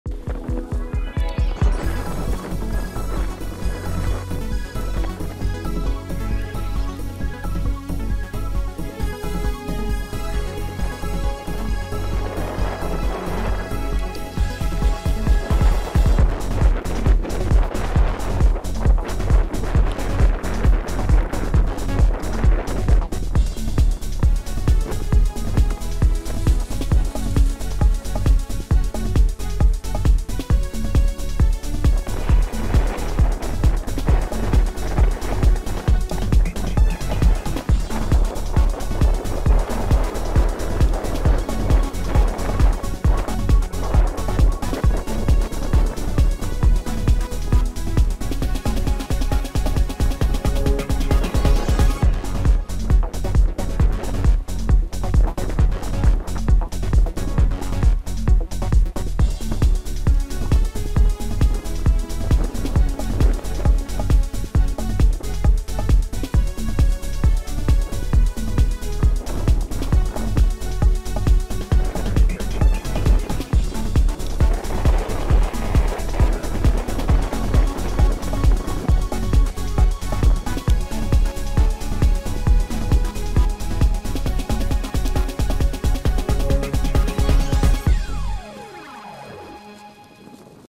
Влог Ижевск в феврале Izhevsk en febrero
КАК МЫ ЖИВЕМ В ИСПАНИИ видео прислал мой сын, Там Настя ходит с телефоном)))))